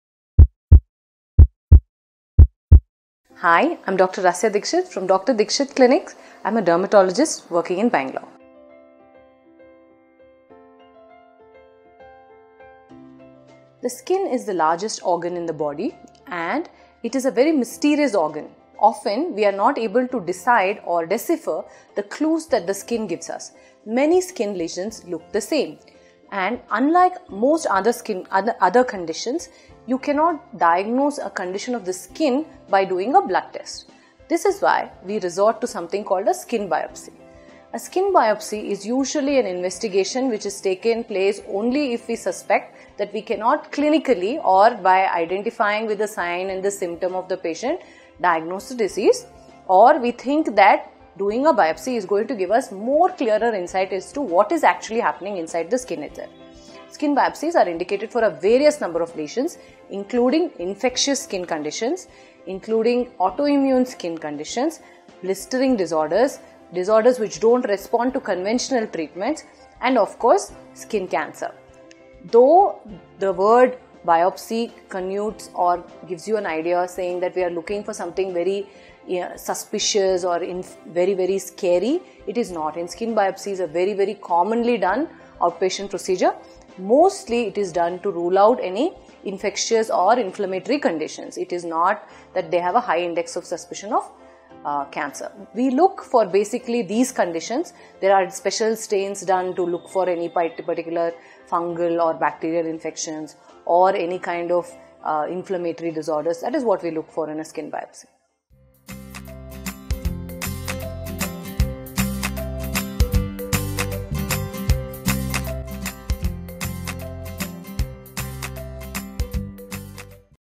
When is skin biopsy planned What do they look for in a skin biopsy? - Dr. Rasya Dixit
The skin is the largest organ in the body and it is very mysterious organ. Often we are not able to decide or decipher the clues the skin gives us. Many skin lesions look the same. Unlike most other skin other conditions, you cannot diagnose a condition of the skin by dong a blood test. This is why we do something called as the skin biopsy. The skin biopsy is usually an investigation which we usually suspect that clinically or by identified by the sign and the symptom of the patient to diagnose the disease or we think that doing a biopsy is going to give a more clear insight as to what is happening in the skin itself. Skin biopsies are indicated for a various number of lesion including infectious skin conditions, including autoimmune skin conditions, blistering disorders, disorders which don’t respond to conventional treatment and of course skin cancer. Though the biopsy connutes or gives you an idea or saying that we are looking for something very suspicious or very very scary. Skin biopsies are very very commonly done patient procedure. Mostly it is done to rule out any infectious or inflammatory conditions. It is not that they have a high index of suspicion of cancer. We look for basically these conditions. There are special stains done to look for any particular fungal or bacterial infections or any kind of inflammatory disorders, that is what we look for in a skin biopsy.